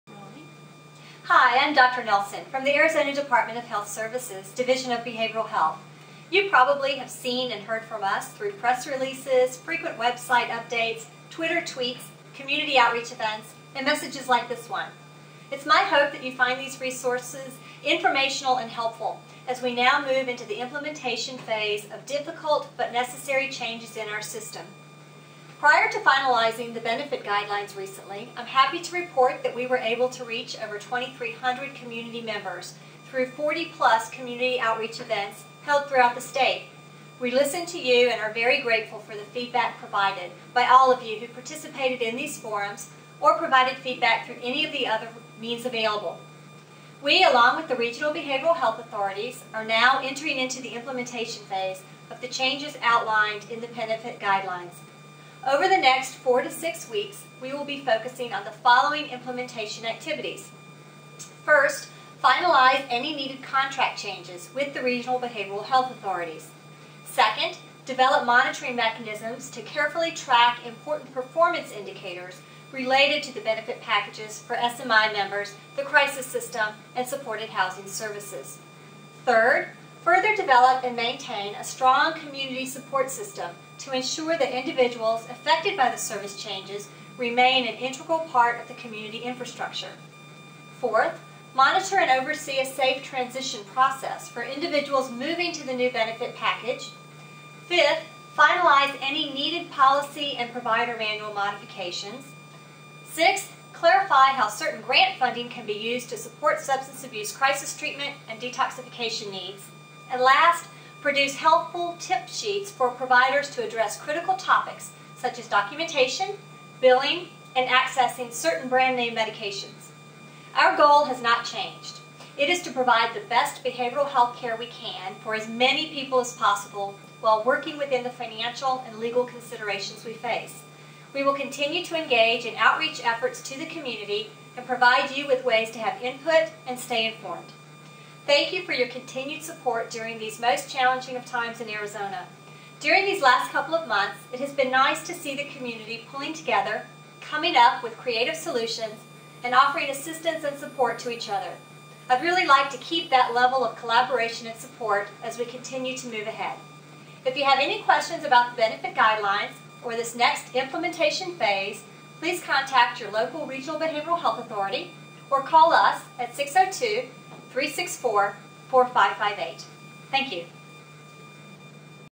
May 21, 2010 - Behavioral Health Services Updates: A Message from Dr. Nelson
Arizona continues to experience a budget crisis.  As a result, deep budget cuts were recently approved that will impact publicly funded behavioral health services.  Our commitment is to continually share updated information with you including through the use of a dedicated Web site, YouTube videos, frequent emails, and numerous community meetings around the state.